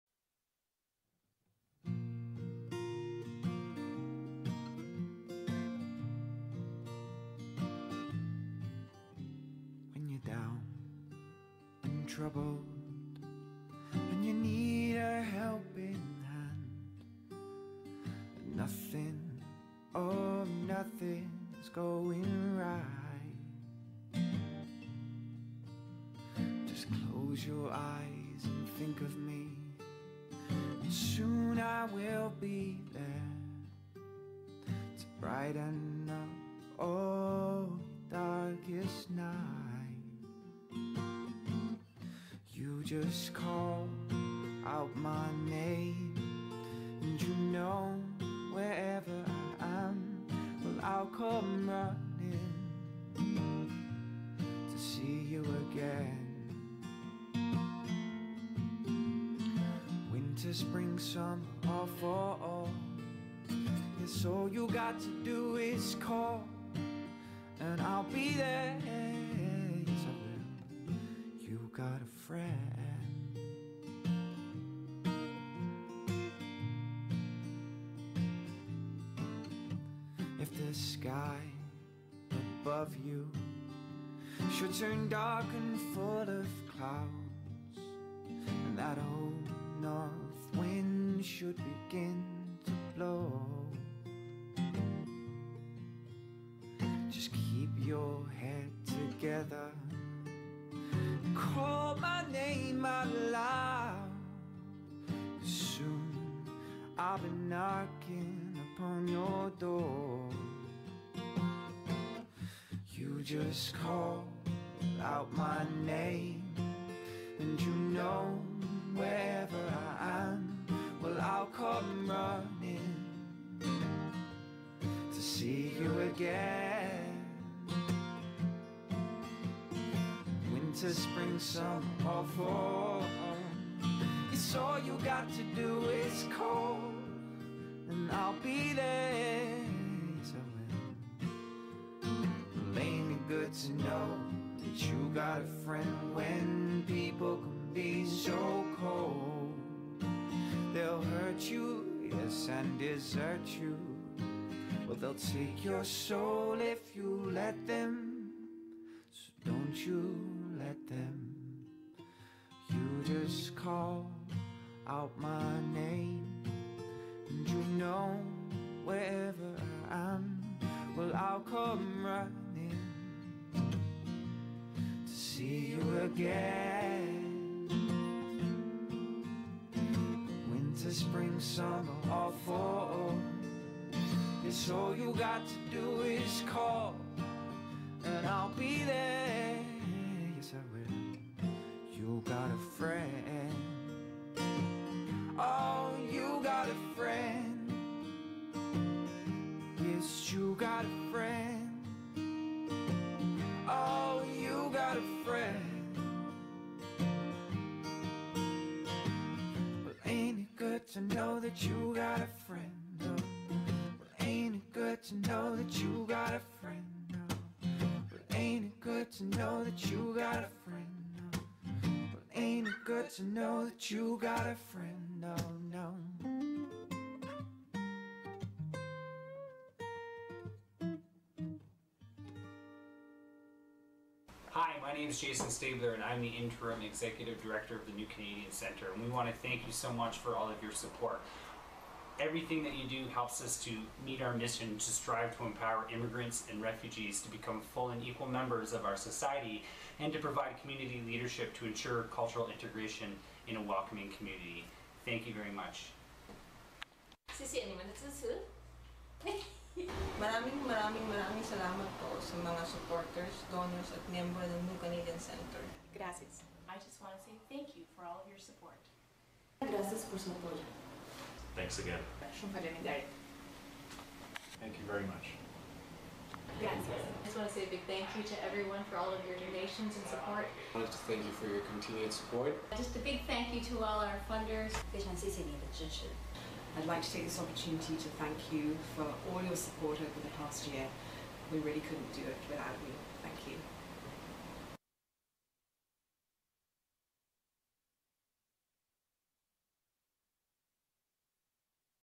New Canadians Centre Annual Report 2012-2013
A review of what we were up to for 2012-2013 and a short message from the staff.